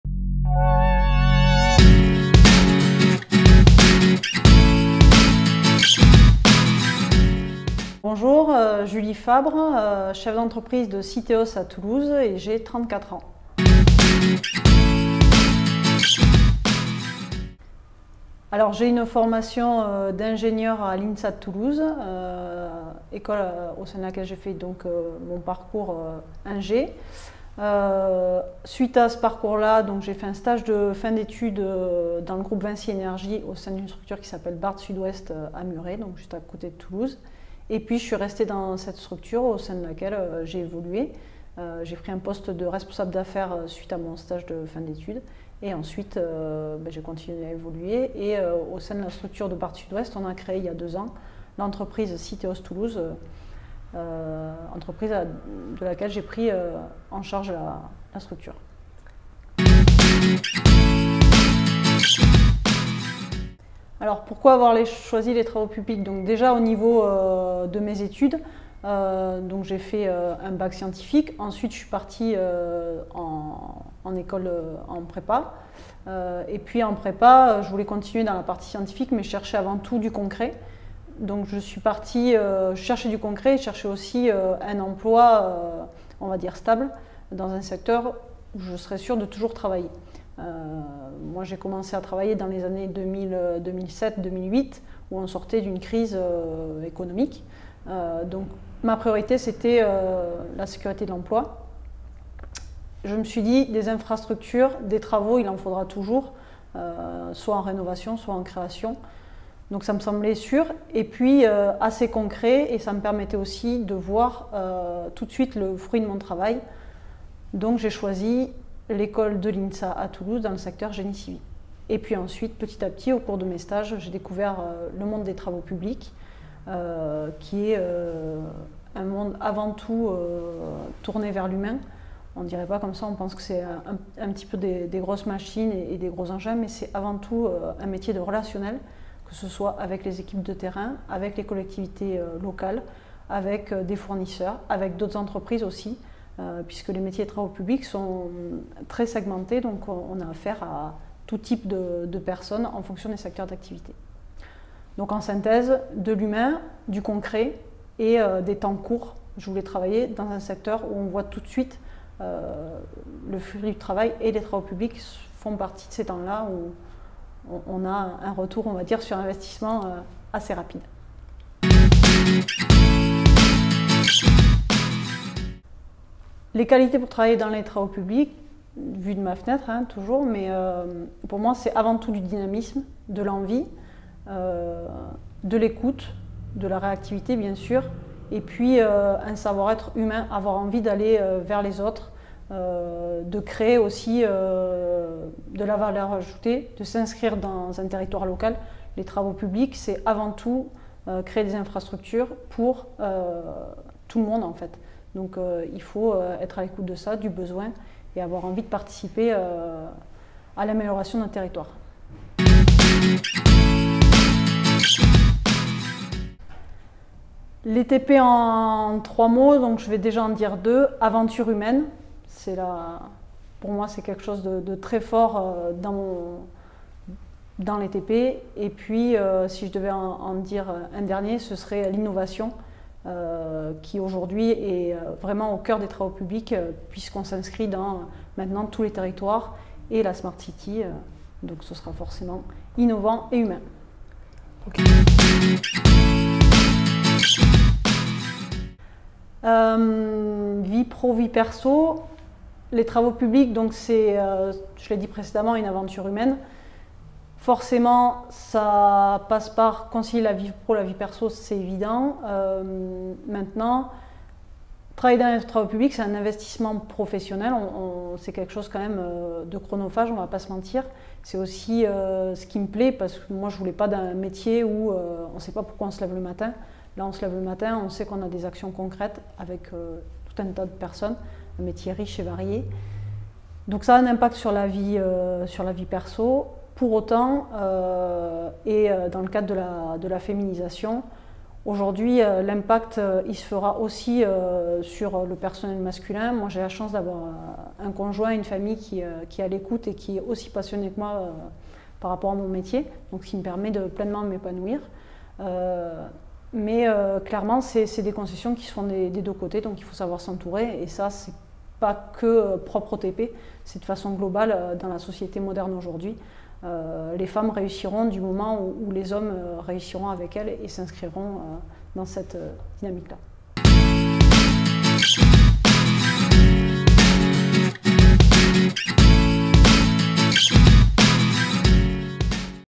Comme Julie, OsezlesTravauxPublics !
Les femmes dans les travaux publics, c’est une réalité ! Comme Julie, OsezlesTravauxPublics !